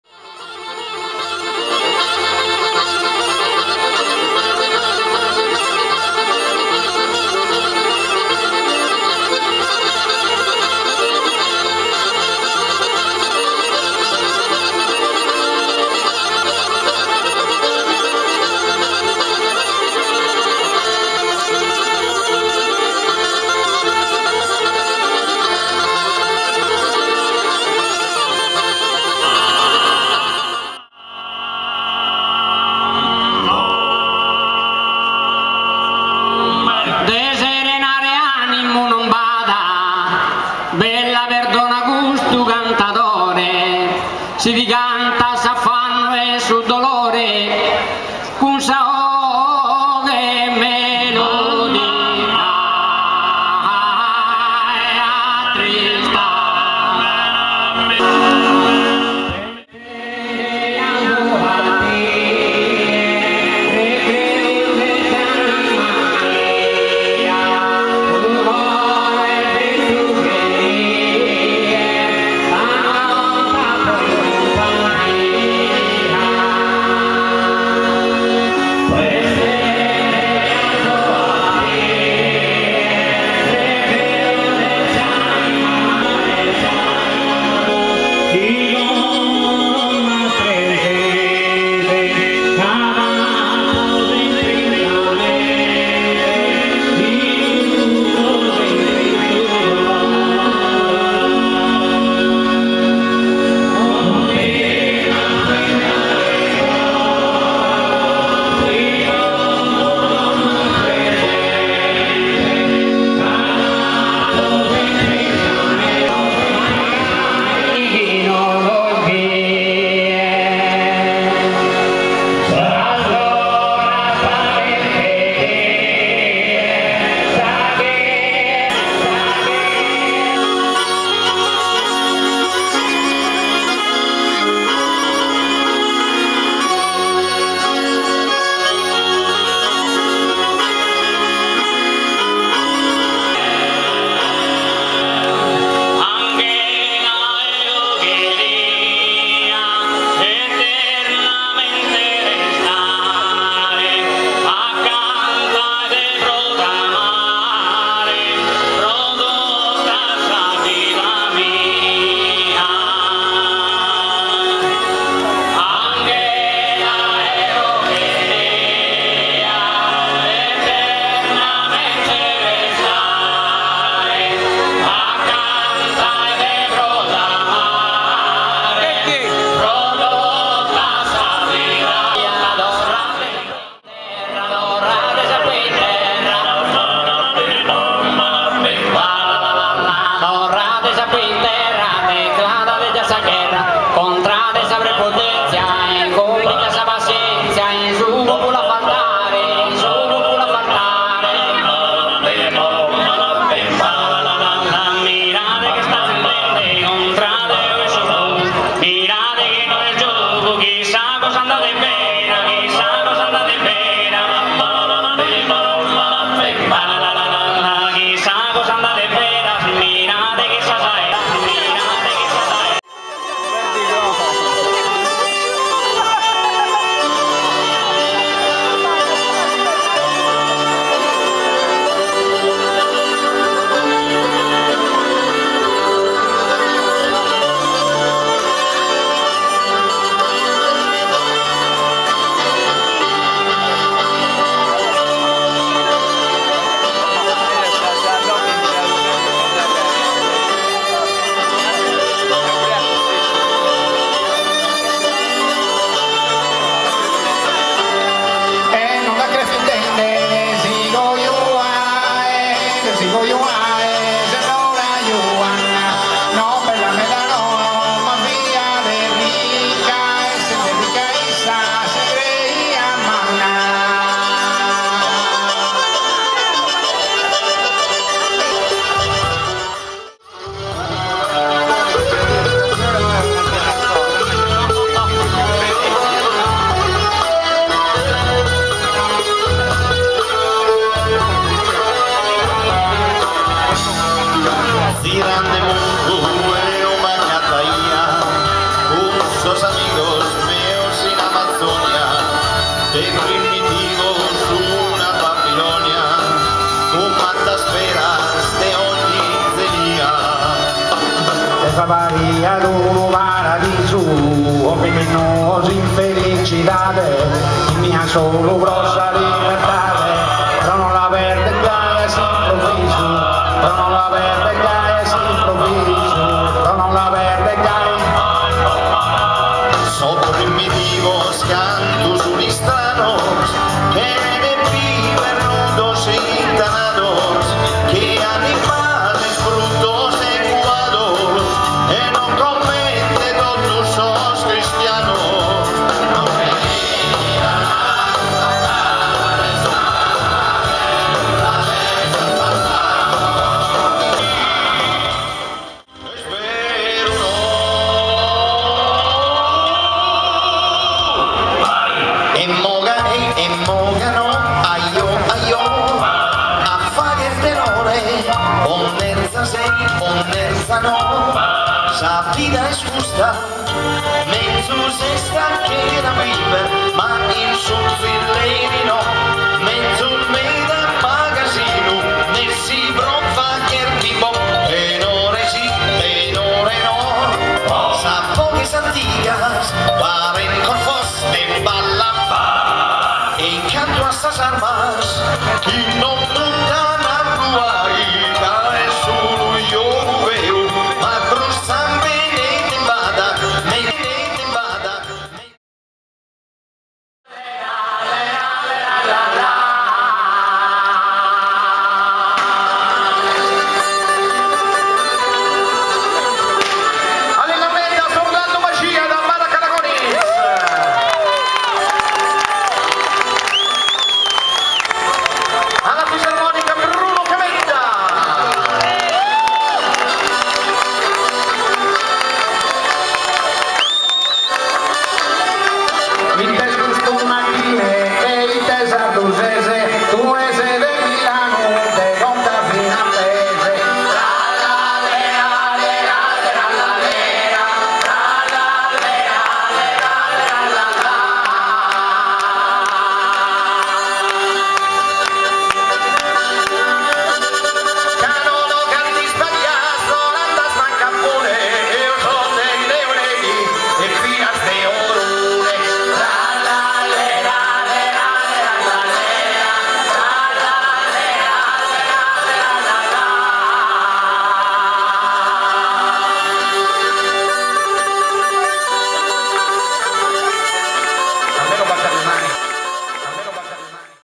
Neonelio, concerto Dolianova 16 luglio 2009
Elio, Tenores di Neoneli, Orlando Mascia e Bruno Camedda
concerto 16 luglio 2009
Dolianova (Ca)